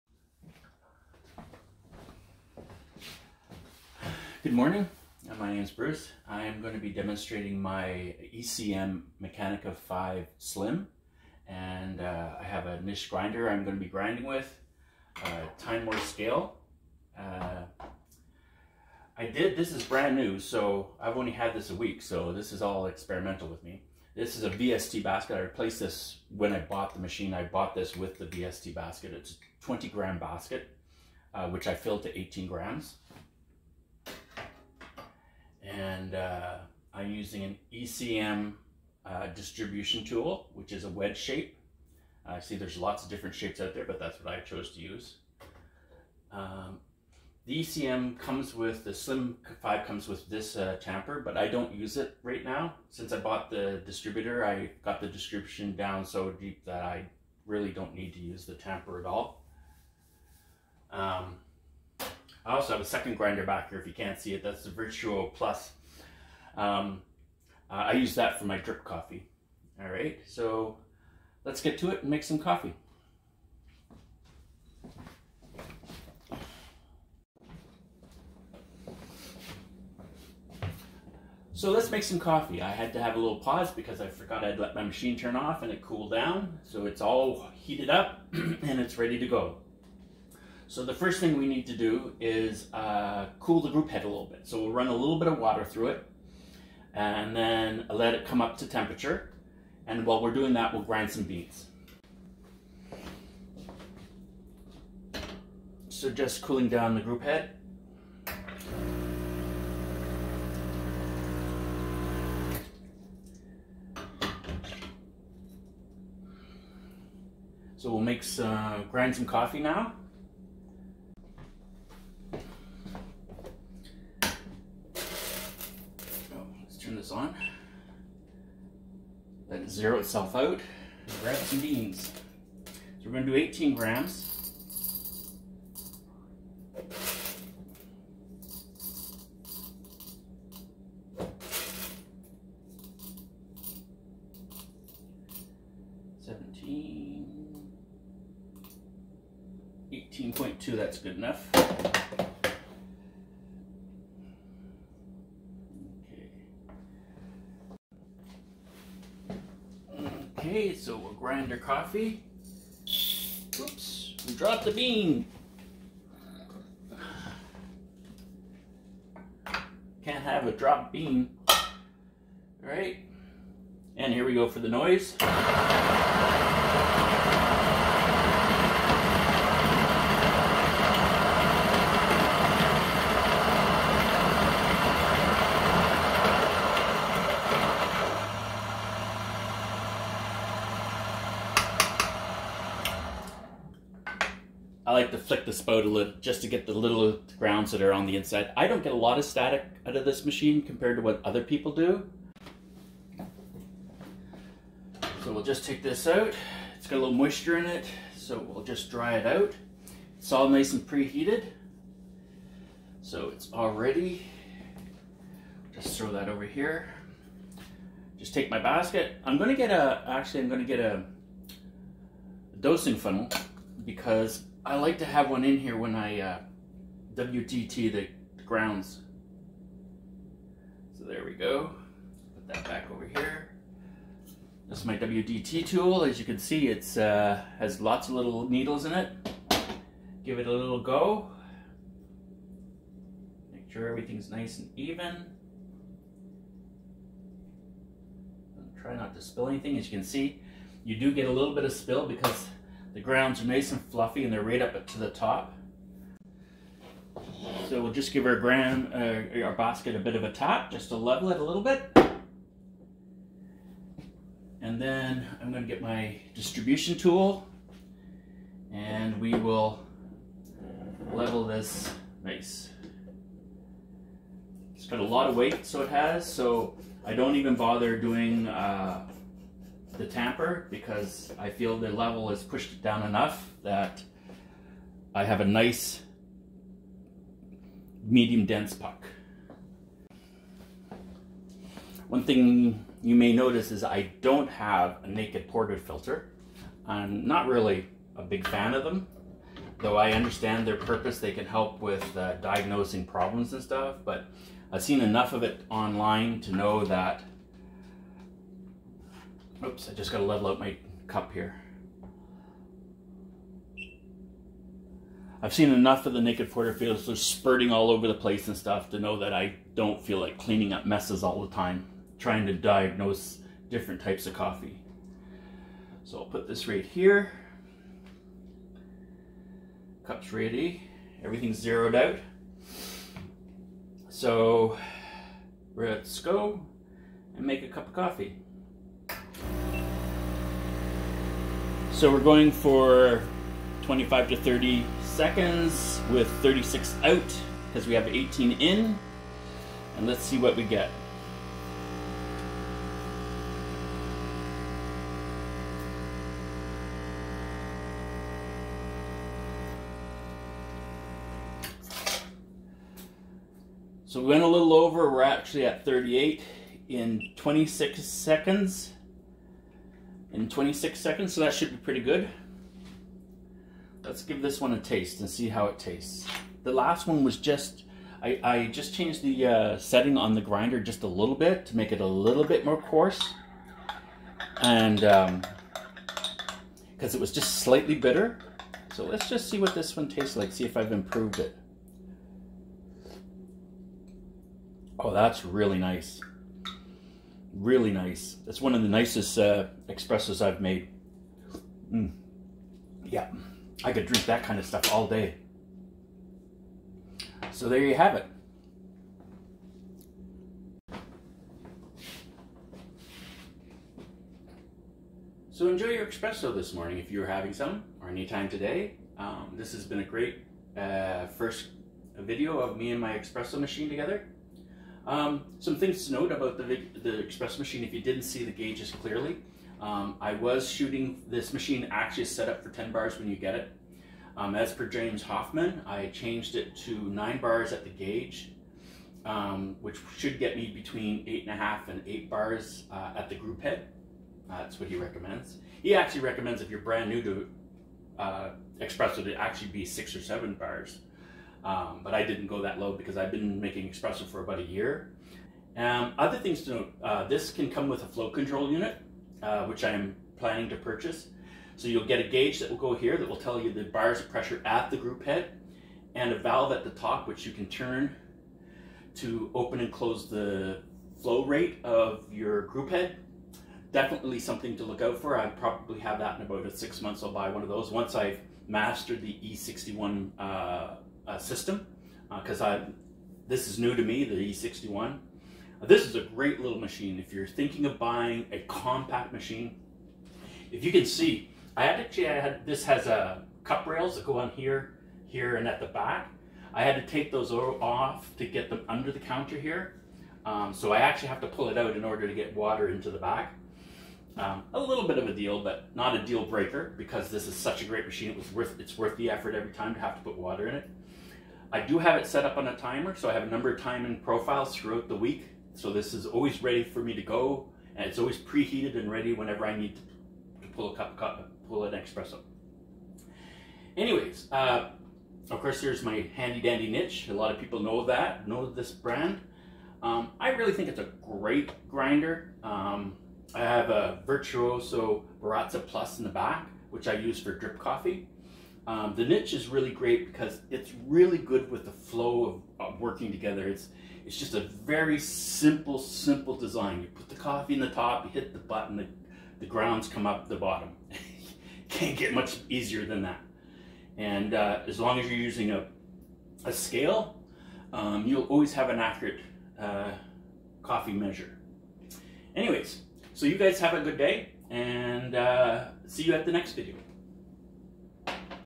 ECM Mechanika V Slim Espresso Machine work flow
New ECM Mechanika V Slim Espresso Machine work flow. This is a new machine I've had just a week. Using 20 gram VST basket and Niche Zero grinder.